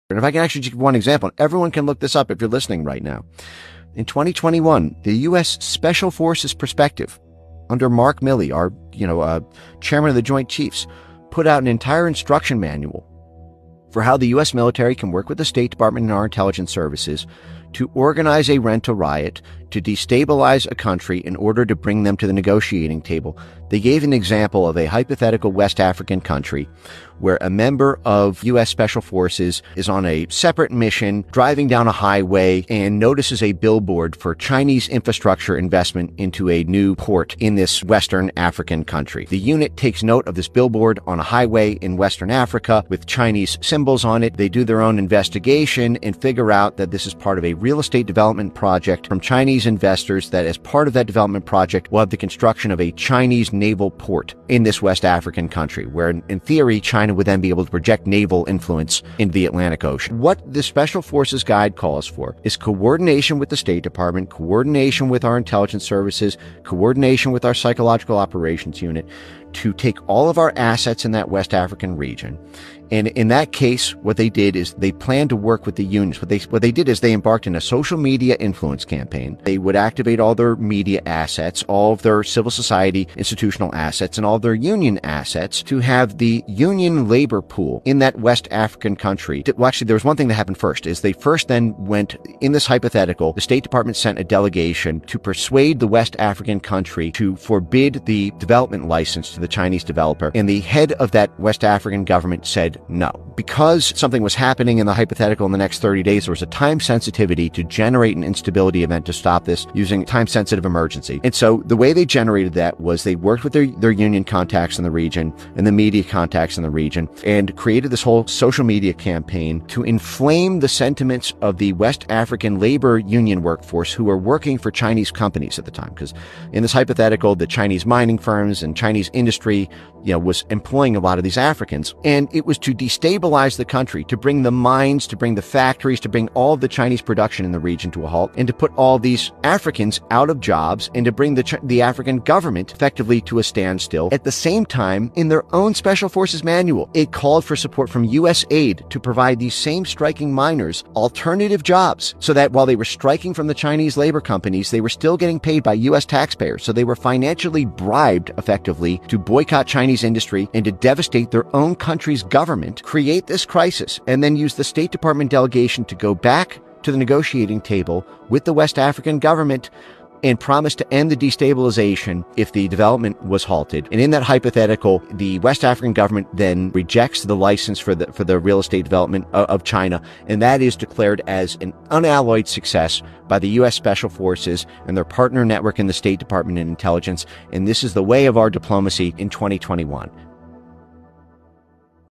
DEEP STATE PLAYBOOK IS NOT JUST A FIGMENT OF IMAGINATION
Deep State Playbook is not just a figment of imagination but an official US Government document.

In 2021, The US Armed Forces put out an Instruction Manual as to how the US Armed Forces can work with the State Department and the Intelligence Services to organise a ‘rental riot’ to destabilise a country in order to bring them to the negotiating table.

This MO may explain a lot of events in India and Bangladesh in recent times as the two countries refused to play ball with US on a number of issues.